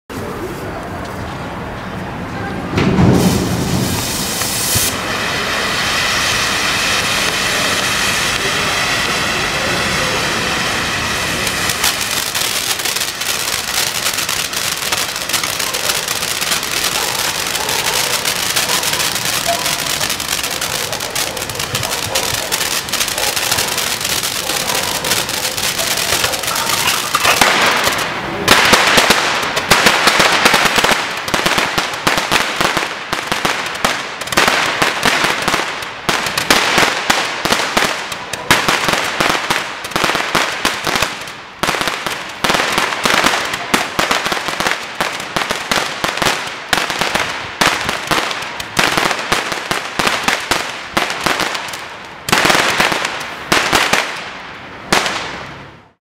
TOMAHAWK 22080
Tomahawk shaped roman candle that emits showers of red and green pearls with silver flowers ending with a finale of crackling chrysanthemums.